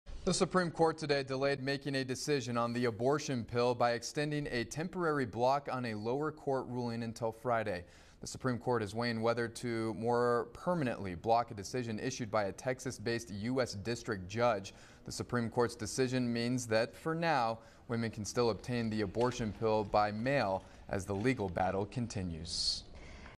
Latest information: Abortion latest
The Supreme Court delayed making a decision on the abortion pill.